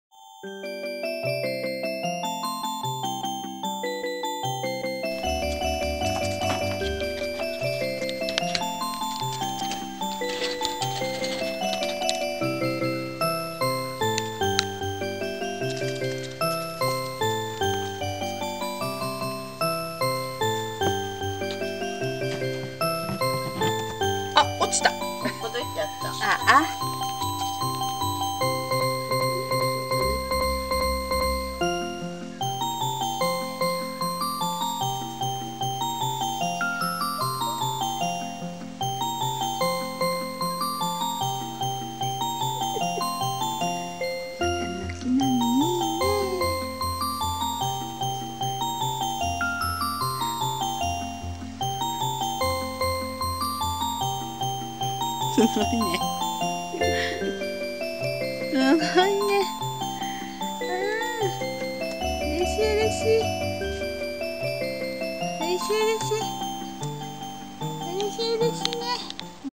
わたの木の実
シマリスふーちゃん 大好きな綿の木の実と格闘中！